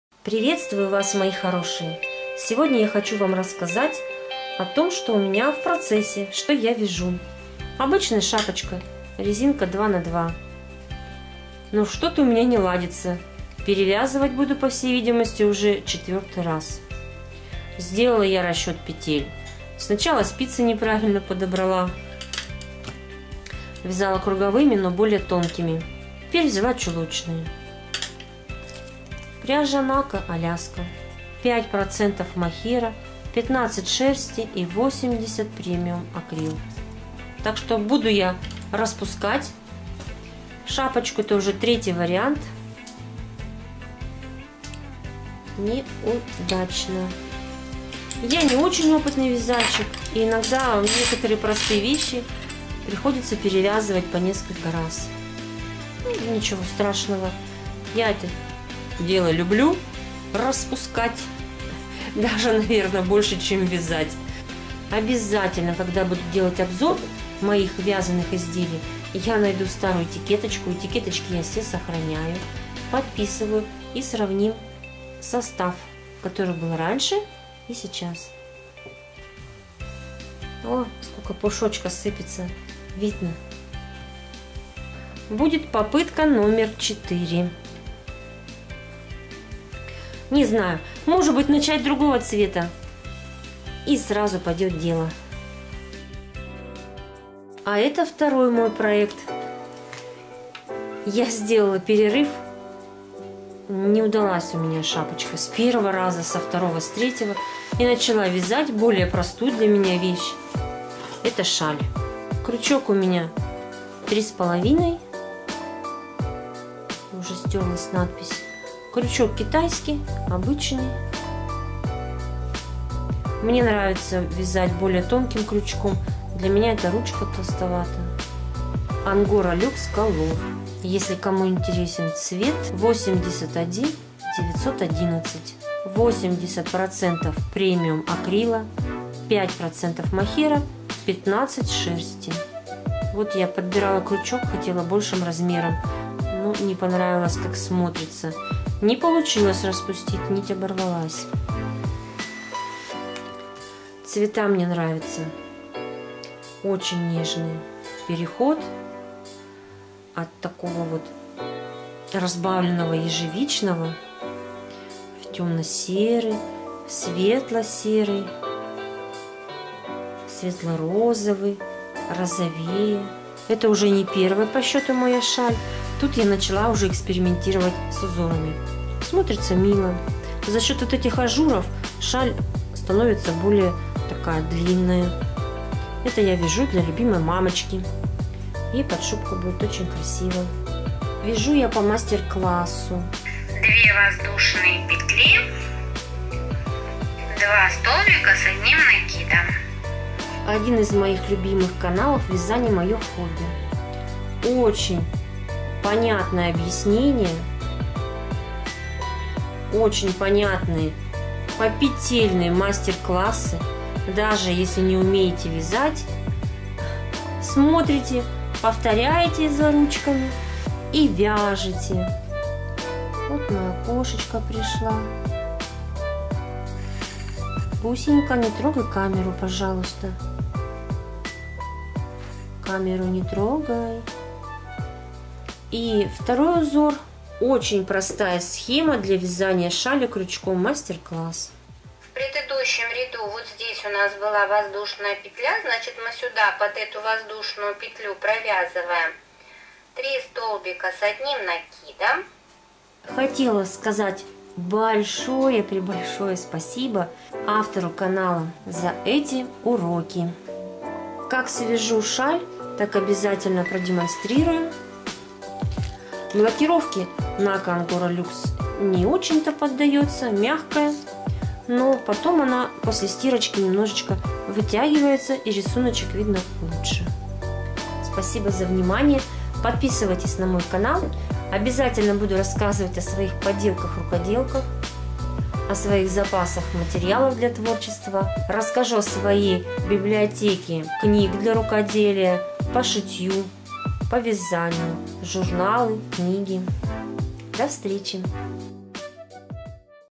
ВЯЗАНИЕ Процессы\Спицы\Крючок (февраль 2021)
Всем привет! В этом видео я покажу,что сейчас вяжу.